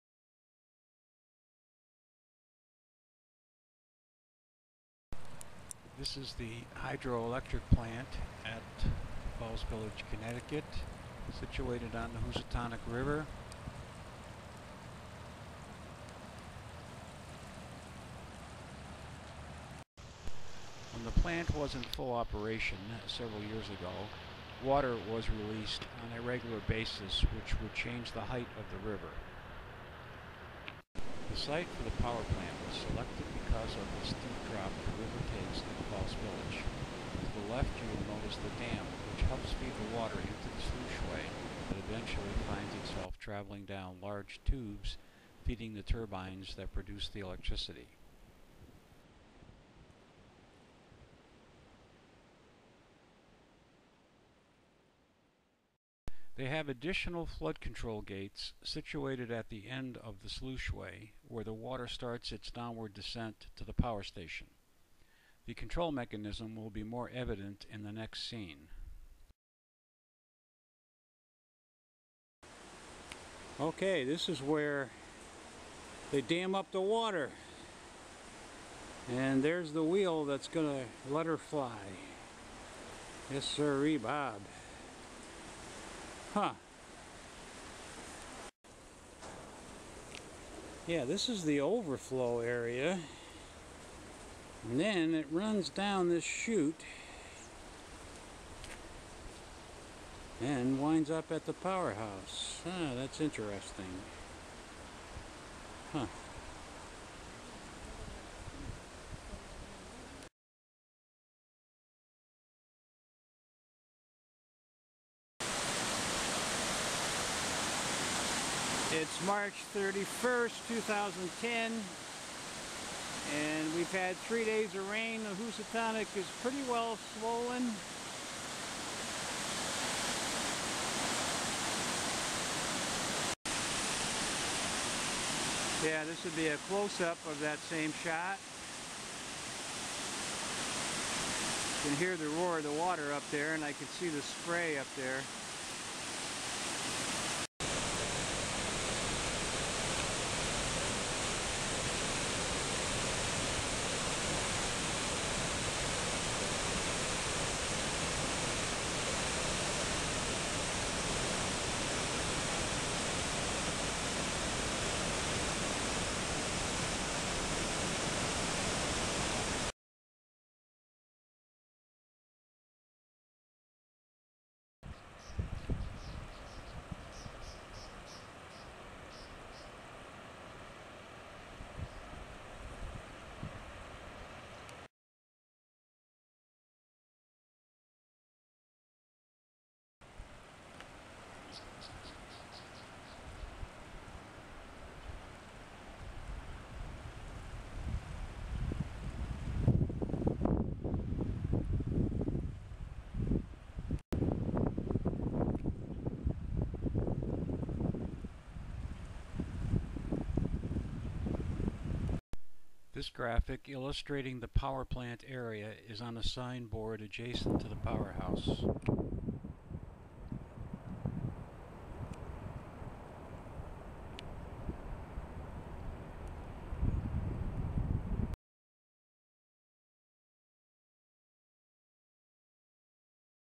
Housatonic River flooding at Falls Village, CT Hydroelectric plant
Description of Hydroelectric facility in Falls Village, CT also shows flooding of the water source during the spring of 2010